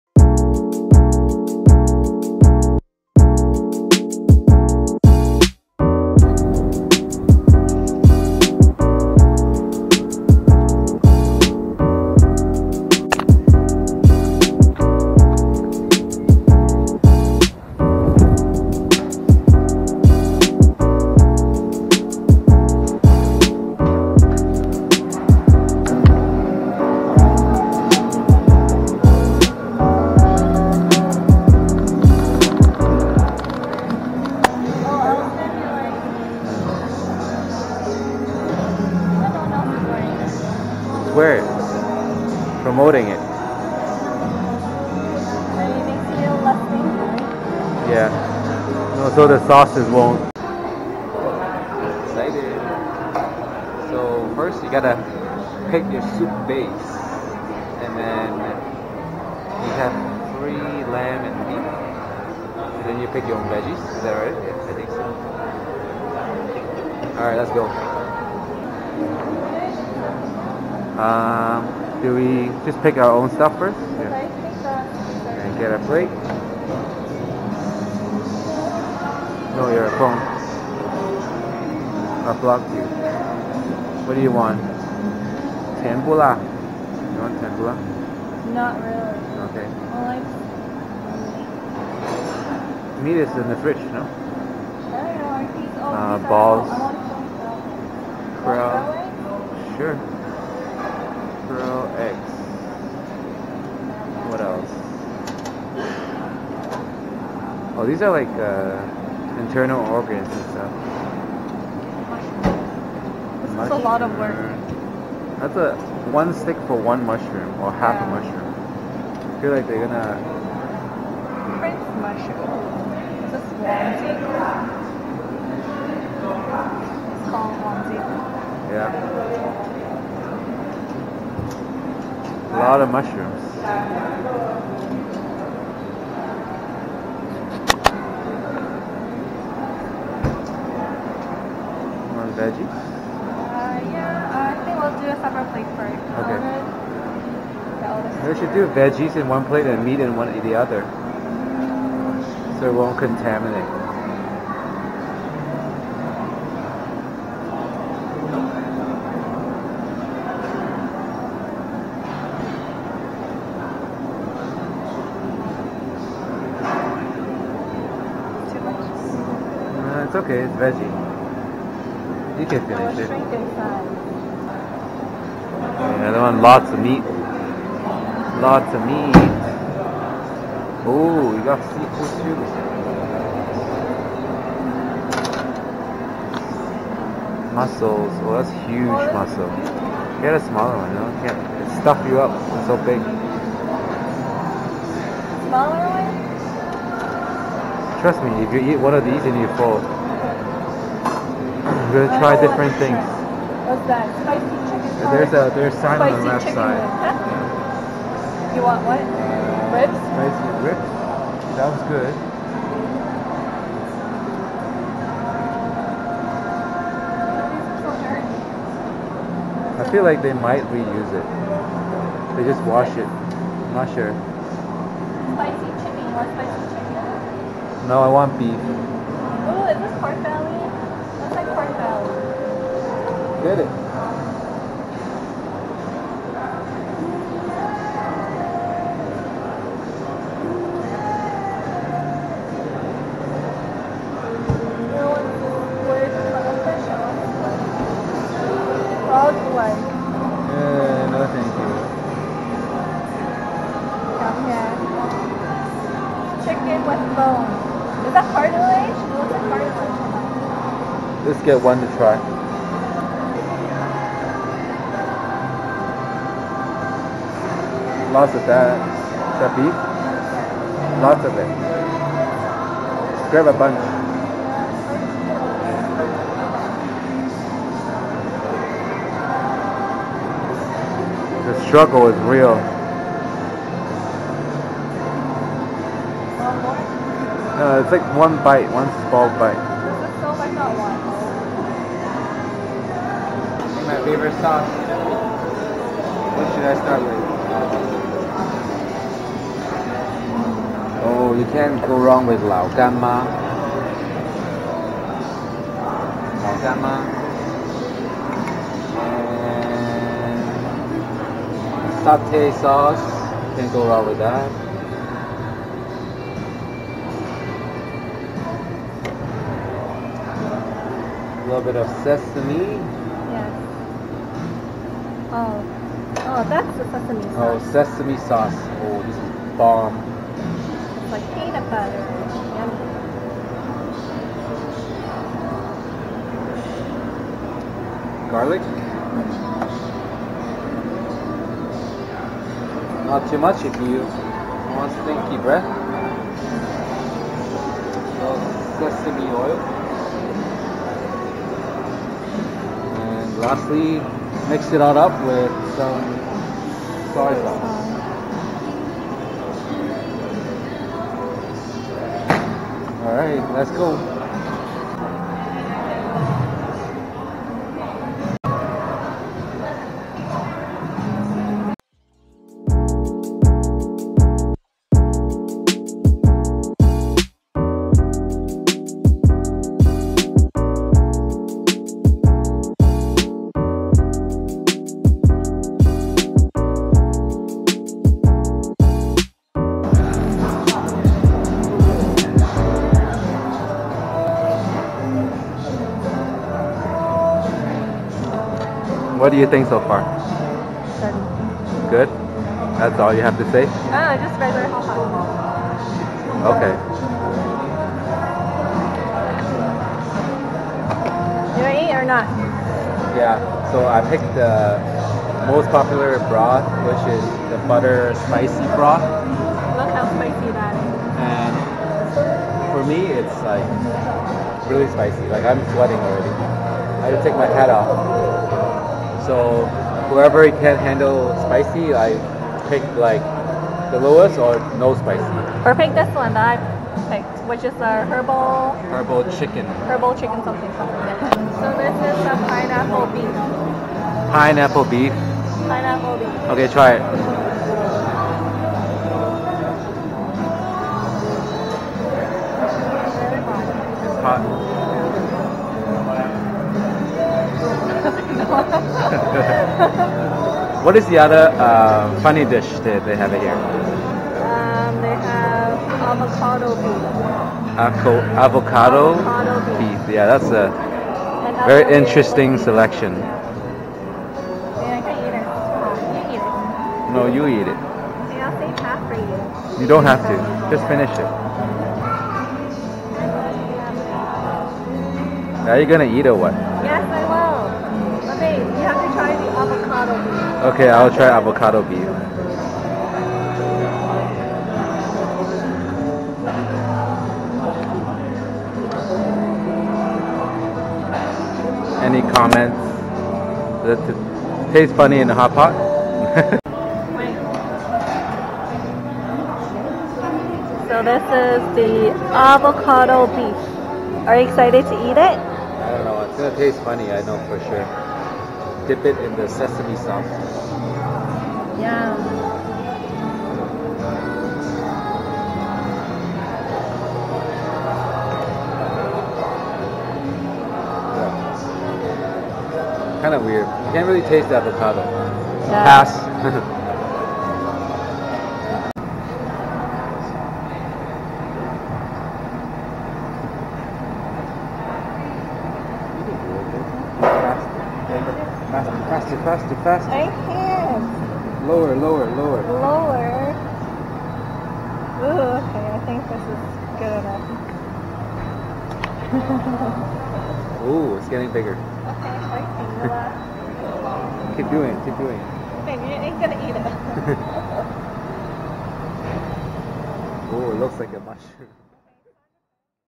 Skewers and hotpot? Why not! (Ye Skewers Hot Pot)
Today, we got to try the new All-You-Can-Eat resto in town, Ye Skewers Hot Pot, located at 3260 Midland Ave F111, Scarborough, ON M1V 0C7.